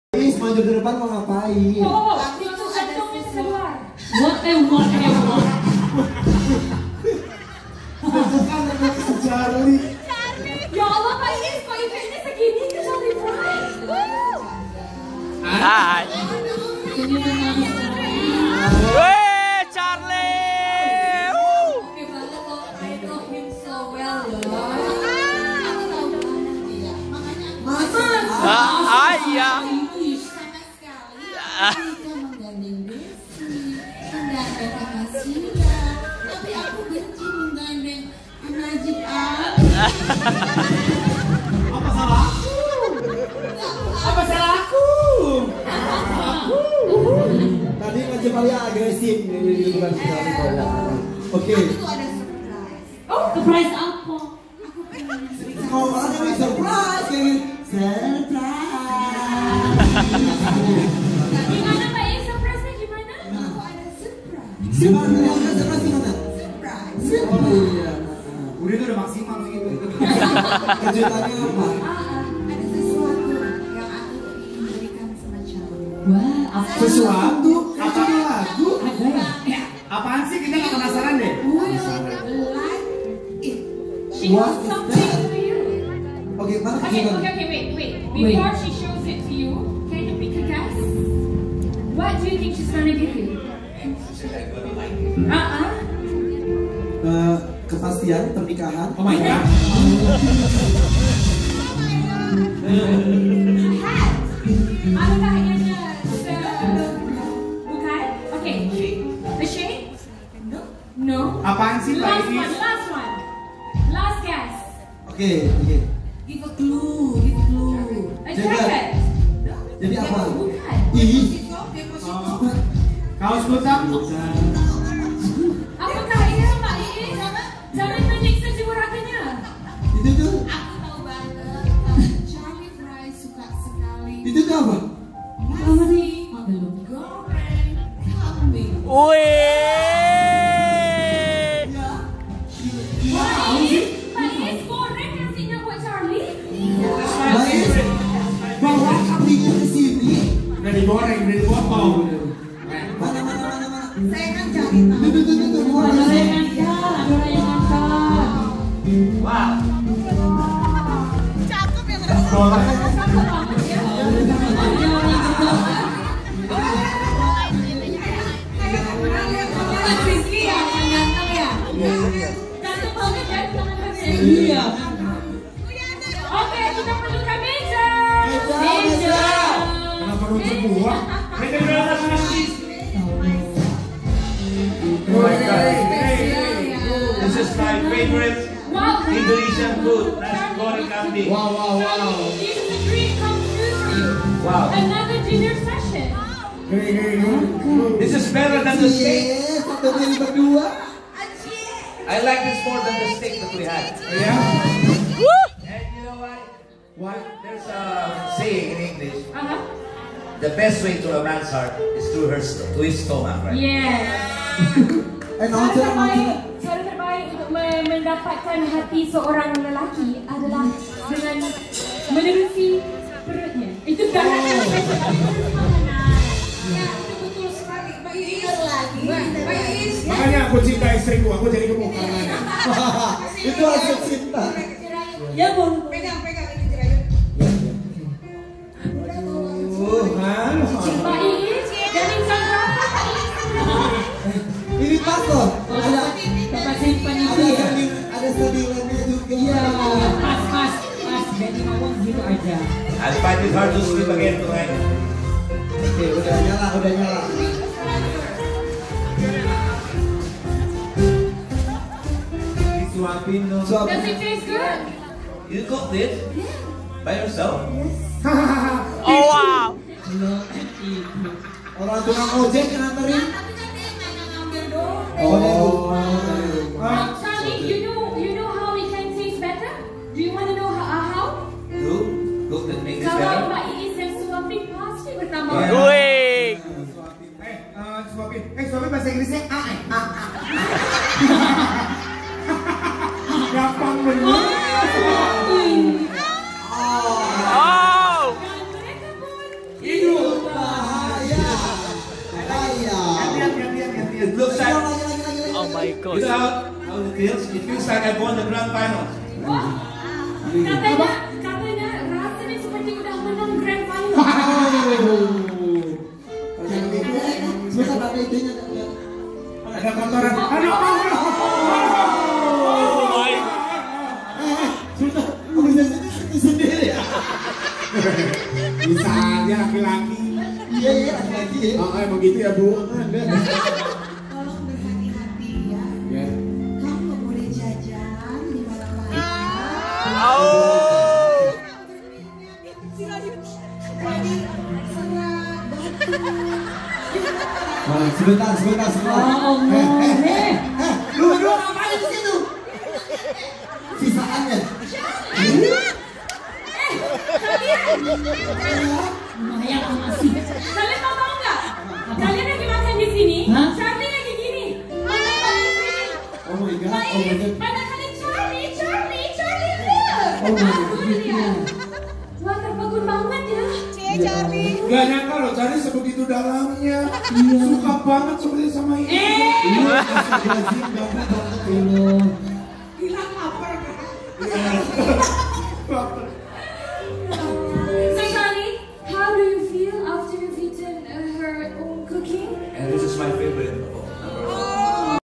SALING SUAPIN!! IIS SUGIANTO & CHARLIE SALING SUAPIN SATU SAMA LAIN, JIRAYUT DAN GILANG IKUT MAKAN
IIS SUGIANTO PASANGAN GIMMICK CHARLIE FRY PALING ROMANTIS. TIDAK HANYA NYANYI BARENG TAPI MAKAN BERSAMA HINGGA SALING SUAPIN... ASIK SEKALI!